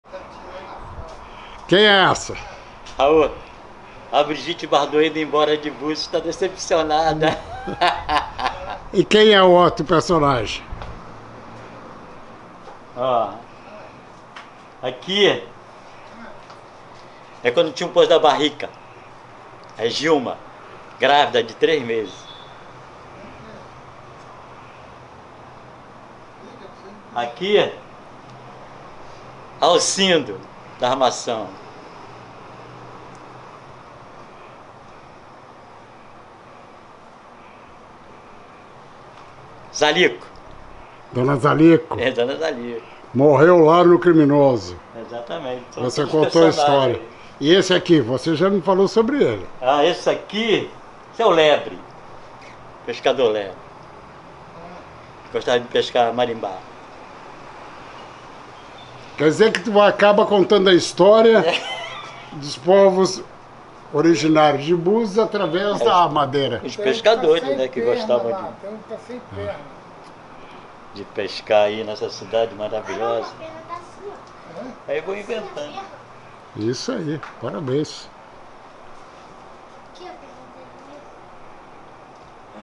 HISTÓRIAS BUZIANAS - CILICO E SUA ARTE NA MADEIRA CONTA HISTÓRIAS DOS PERSONAGENS DA VIDA EM BÚZIOS
Um dos buzianaos mais conhecidos de Búzios é o Cilico, de uma família tradicional na cidade, os Murebes, ele foi dono de um restaurante em Manguinhos e atualmente faz trabalhos com madeira,